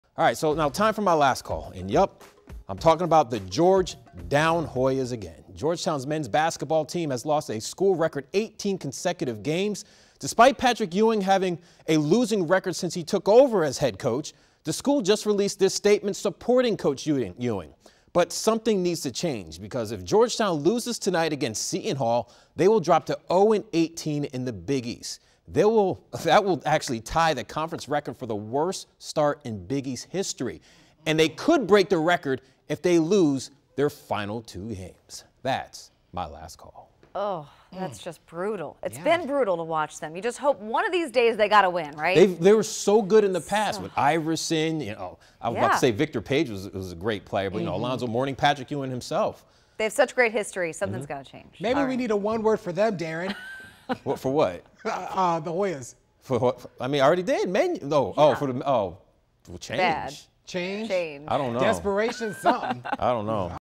Georgetown basketball loses 18 consecutive games
Things are not looking good for the Georgetown Hoyas men's basketball team.